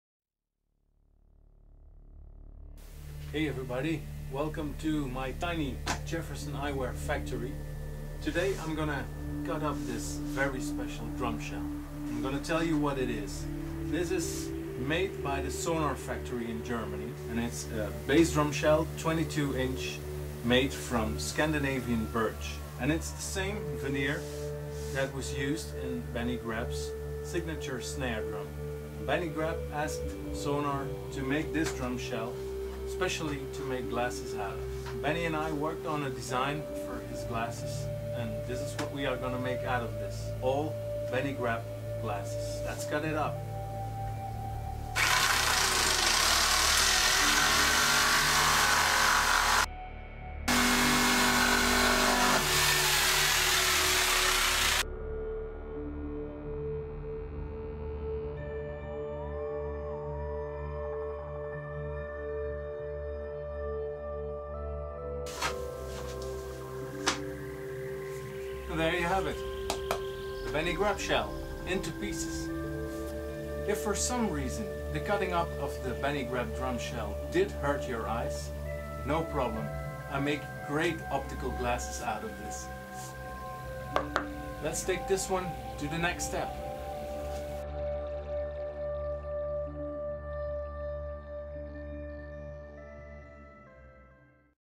Cutting up the "Benny Greb" shell
Sonor drum co. made a drum shell from the Scandinavian Birch veneer that's been used for the Benny Greb signature snare drum. They made this for me to make glasses from, designed by Benny and me.